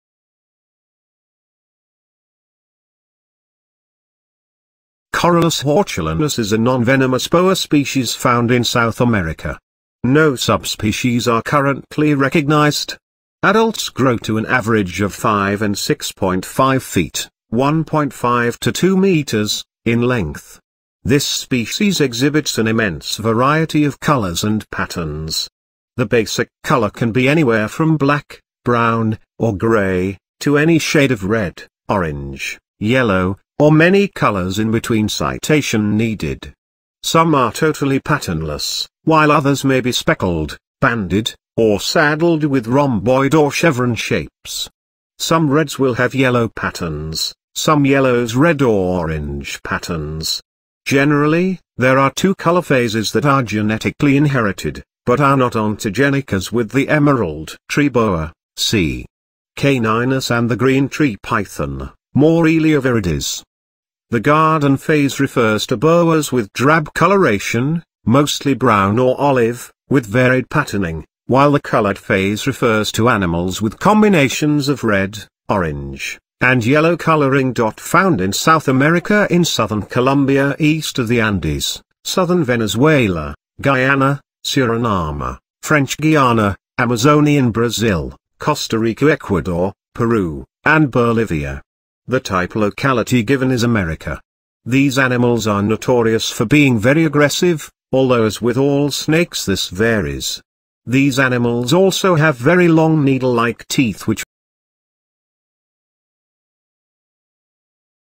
Interesting facts about Boa cock's tree by weird square
Corallus hortulanus is a non-venomous boa species found in South America. No subspecies are currently recognized.
Adults grow to an average of 5 and 6.5 feet (1.5–2 m) in length. This species exhibits an immense variety of colors and patterns. The basic color can be anywhere from black, brown, or gray, to any shade of red, orange, yellow, or many colors in between[citation needed]. Some are totally patternless, while others may be speckled, banded, or saddled with rhomboid or chevron shapes. Some reds will have yellow patterns, some yellows red or orange patterns. Generally, there are two color 'phases' that are genetically inherited, but are not ontogenic as with the emerald tree boa,C. caninus and the green tree python, Morelia viridis. The 'garden phase' refers to boas with drab coloration, mostly brown or olive, with varied patterning, while the 'colored phase' refers to animals with combinations of red, orange, and yellow coloring.Found in South America in southern Colombia east of the Andes, southern Venezuela, Guyana, Suriname, French Guiana, Amazonian Brazil, Costa Rica Ecuador, Peru, and Bolivia. The type locality given is "America."
These animals are notorious for being very aggressive, although as with all snakes this varies. These animals also have very long needle-like teeth which makes their bite quite painful. However these snakes tend to give some warning of being inclined to bite, and will usually give fairly gentle bites (which can still draw blood) unless they are given reason to give a full strike.
A more preliminary attempt to dissuade a potential attacker, a tree boa will whip its tail and release a foul smelling liquid that is difficult to remove. This is a similar tactic seen in different animals from skunks to insects. Animal planet | Discovery | Netflix | Documentary | Animal World